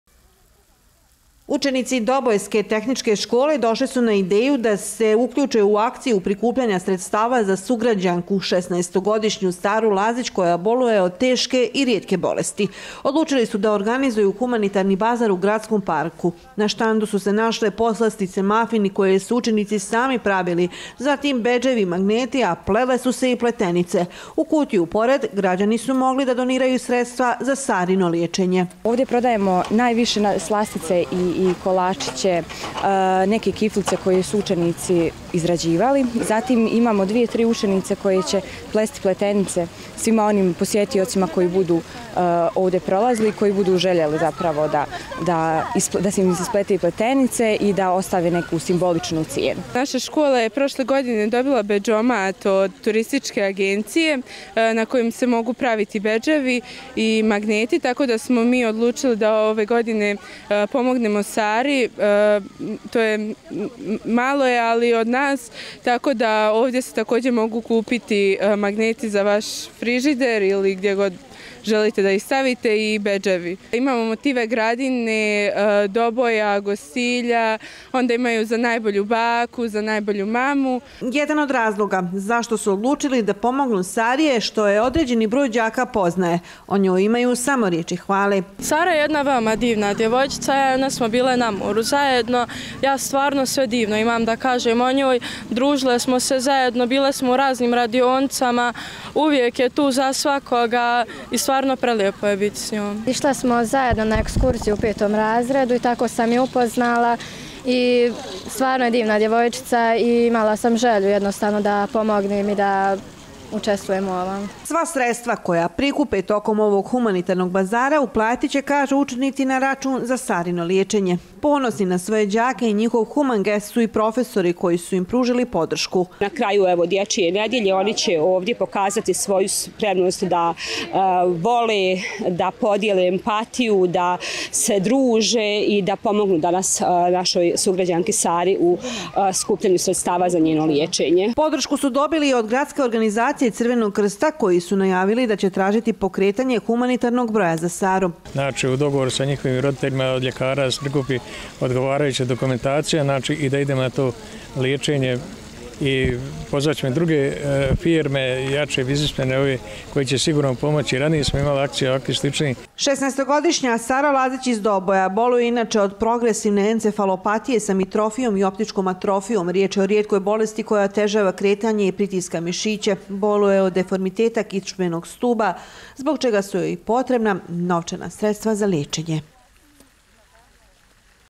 DOBOJ: HUMANITARNI BAZAR
U AKCIJI ZA PRIKUPLJANJE SREDSTAVA ZA LIJEČENJE SARE LAZIĆ, DJEVOJČICE IZ DOBOJA, KOJA BOLUJE OD RIJETKE I TEŠKE BOLESTI, STIŽE PODRŠKA UČENIKA TEHNIČKE ŠKOLE IZ OVOG GRADA. ZAJEDNO SA GRADSKOM ORGANIZACIJOM  CRVENOG KRSTA ORGANIZOVALI SU HUMANITARNI BAZAR, A SVA SREDSTVA PRIKUPLJENA OD PRODAJE UPLATIĆE UPRAVO NA RAČUN ZA SARINO LIJEČENJE.